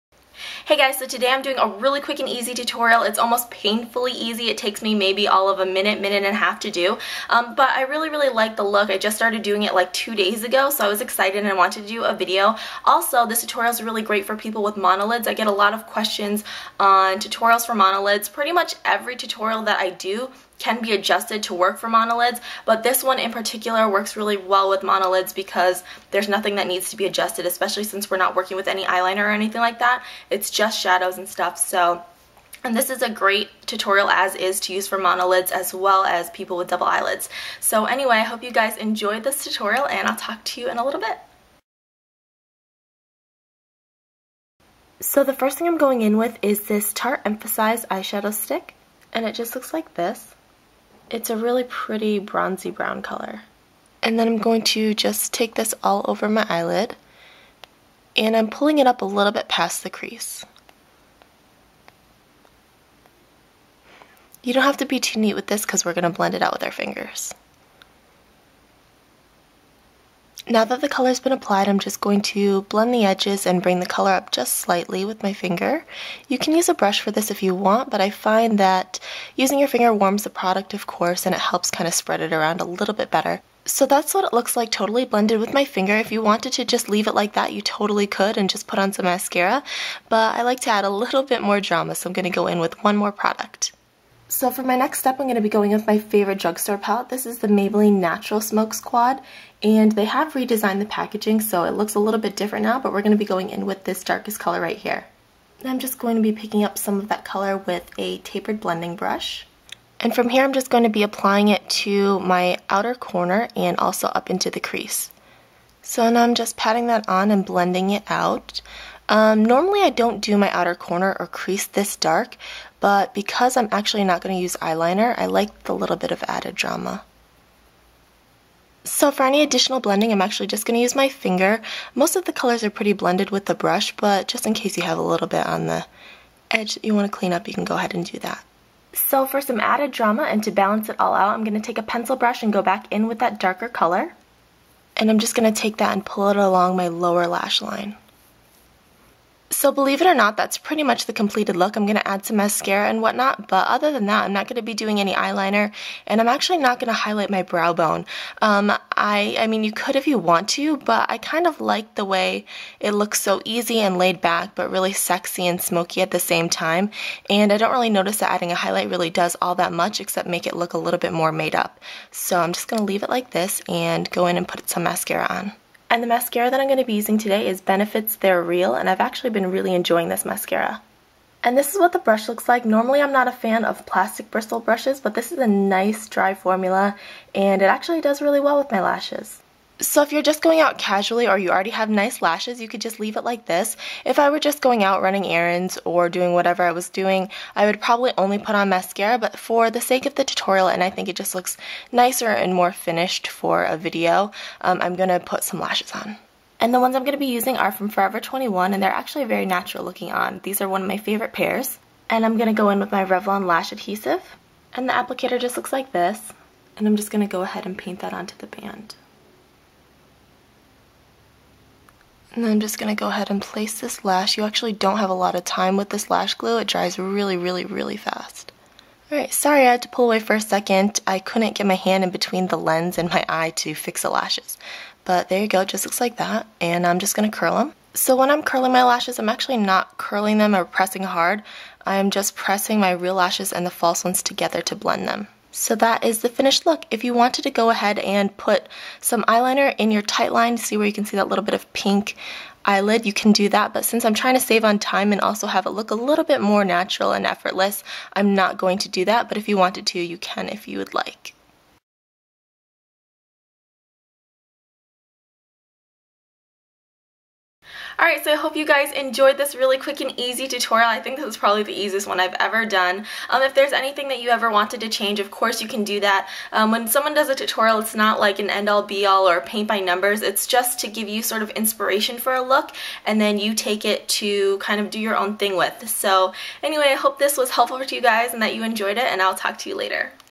2 Minute Smokey Bronze - Quick Everyday Eye Tutorial (Great for MONOLIDS Too!)
*** if you dont know what monolids are please GOOGLE them vs. post a million comments on it. google will be a much better resource. ALSO I DO NOT HAVE MONOLIDS (i have one monolid and on double but when you see me in my videos I have 2 small DOUBLE lids)

ALSO I DO APPLY A BIT OF MASCARA BEFORE THE TUTORIAL SOLELY FOR FILMING OTHERWISE MY LASHES DONT SHOW UP ON CAMERA

PRODUCTS USED:
Tate Emphaseyes Shadow Stick (I dont see a color on it, it was in a kit I bought from Sephora)
Maybelline Stylish Smokes Natural Smokes Quad
Mascara - Benefit Theyre Real (Please note that almost all mascaras look the same on my short lashes and that you are also seeing them unnaturally close.)
Lashes: Forever 21 (i think these are number 81)

What else I'm Wearing:
Face: Missha Perfect Cover BB Cream #23
MAC Studio Careblend Powder in Medium
NYC Sunny Bronzer
MAC Beauty Powder in Oh so Fair (VV collection)
Maybelline Age Rewind Double Ended Concealer
Lips:
NARS Cruising Sheer Lipstick
NARS Turkish Delight Lipgloss

Shirt is Forever 21

Nails - I'm not sure, I got them done this time and there was no sticker at the bottom. Its OPI though. Looks like Rumples Wiggin' (it was limited edition i believe) otherwise Essie Nice is Nice looks kind of close, a little more grey.